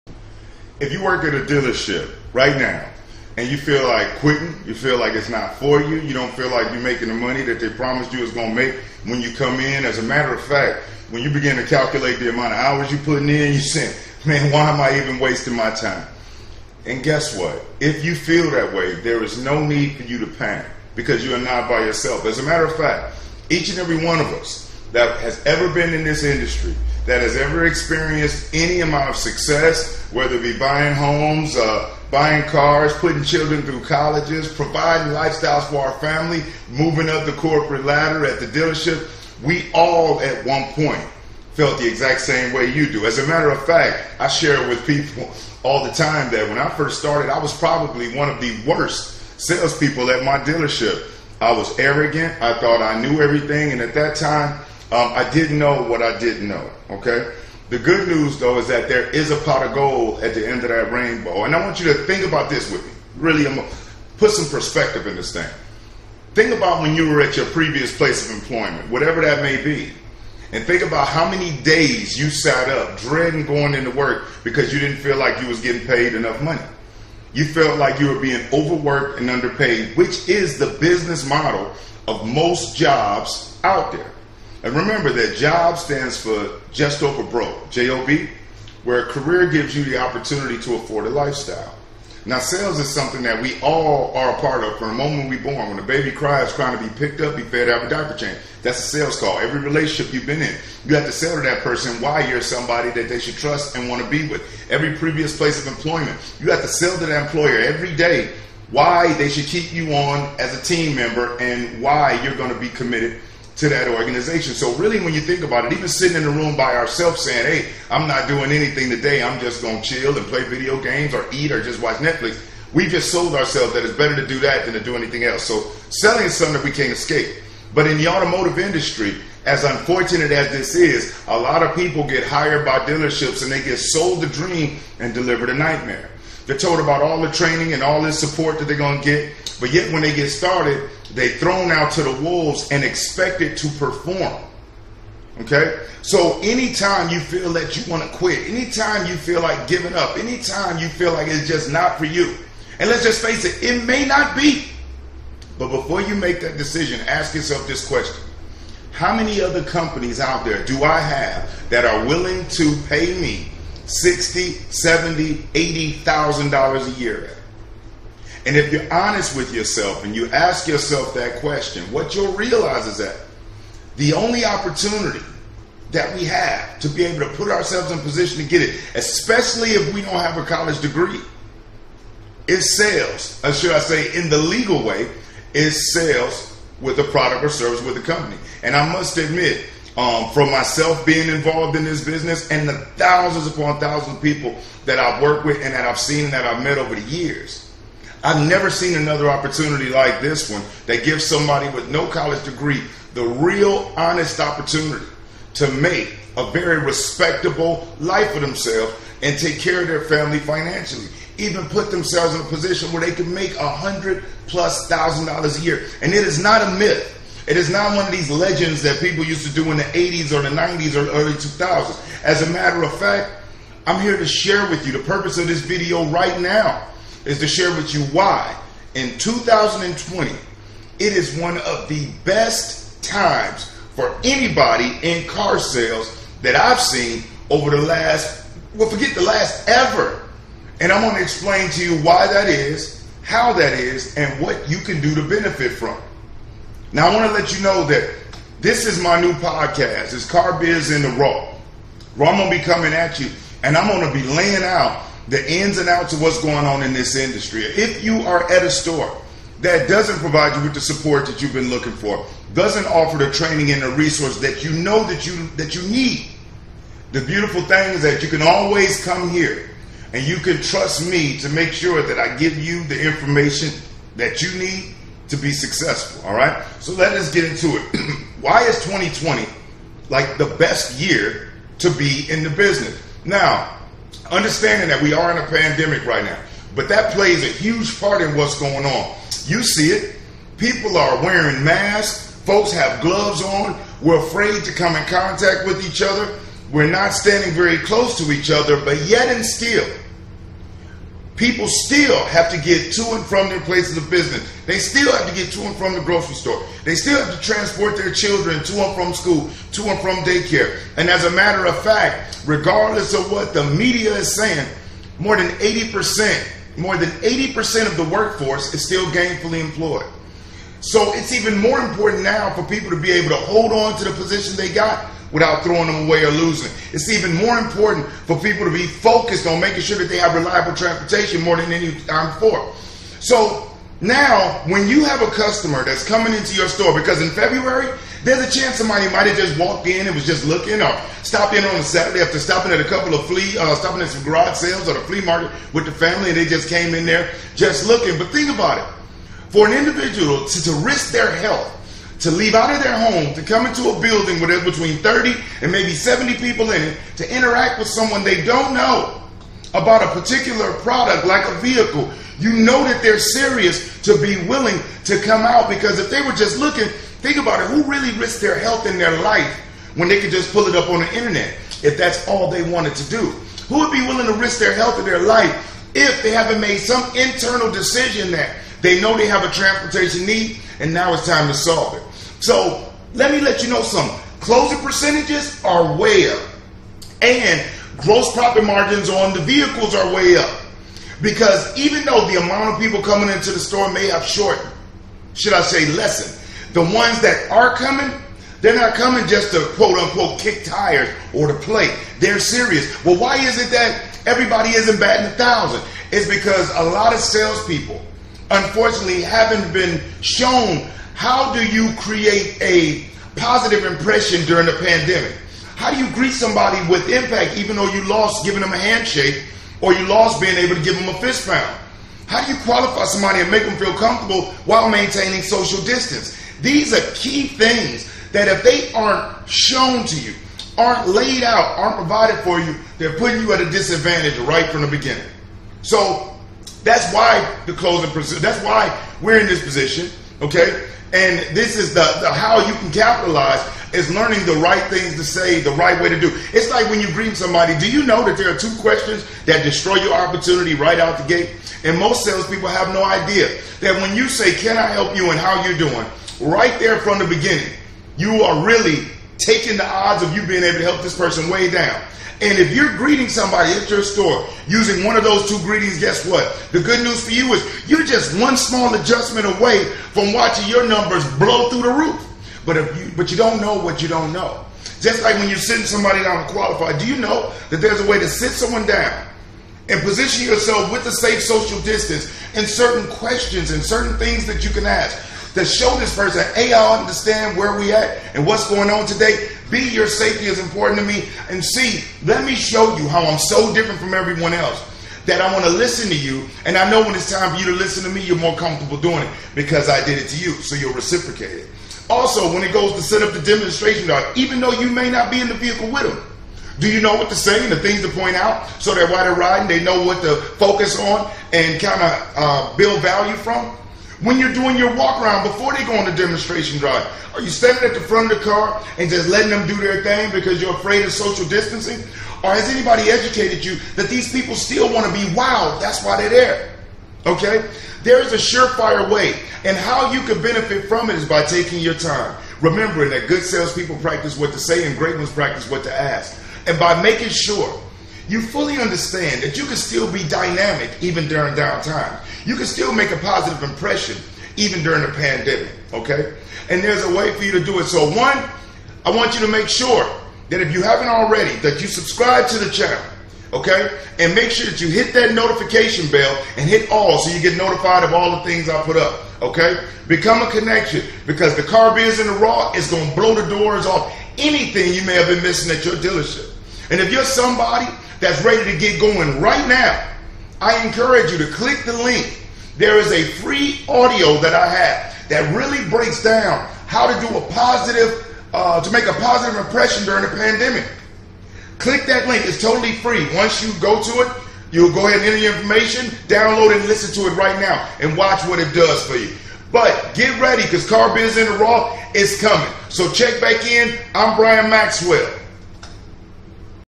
Here's Why 2020 is a Great Year to be in Car Sales
Here's a BIG reason 2020 is turning out to be a much better year for car salesman than expected. Despite social distancing guidelines here's why car salesman who perform professionally and have a winning sales process are having success at car dealerships nationwide.

Download the FREE audio How to Make a Positive Impression During a Pandemic